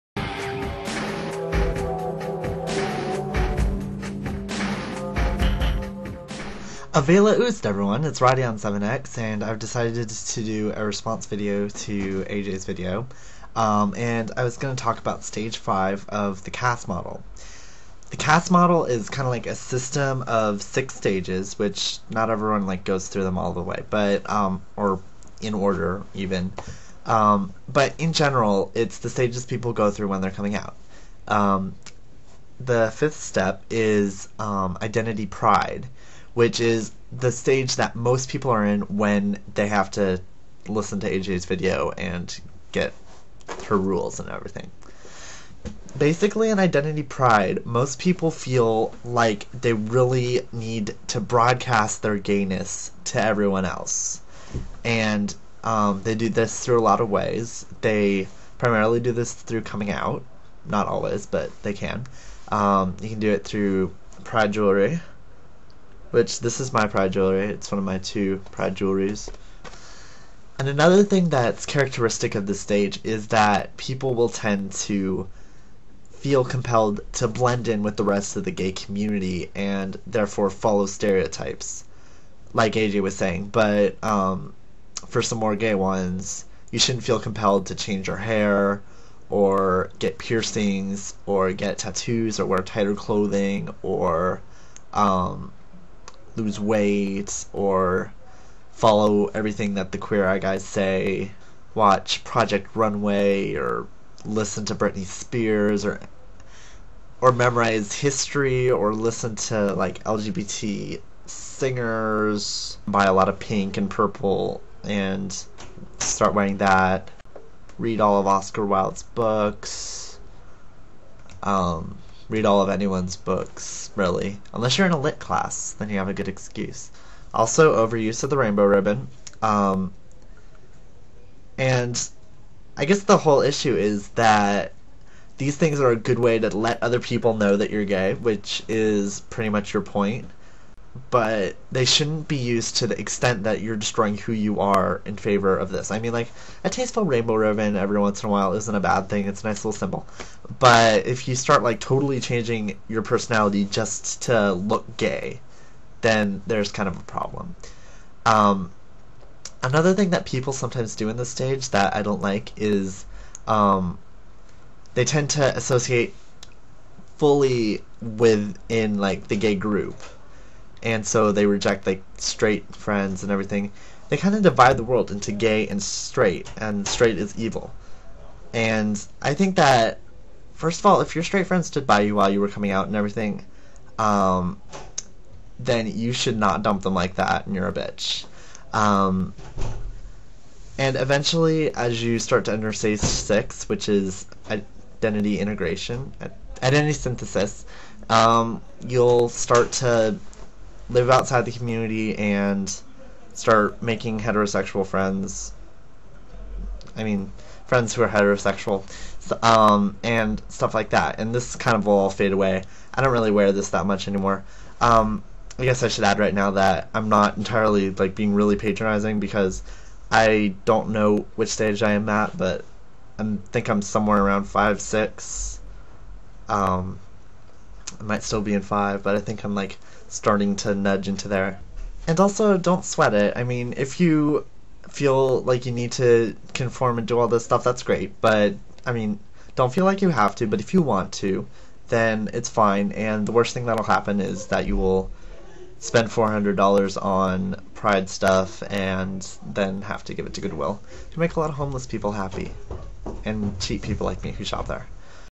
Stage 5: Identity Pride
I discuss that time in many a queer youth's life when they feel the need to let everyone know that they are, in fact, a queer youth. Some never move on...

Now that I think about it, my girlfriend and I don't seem to agree on a whole lot, do we?

Yeah, I upped the brighness a bit too much, the room was sorta dark or the camera was sorta not working...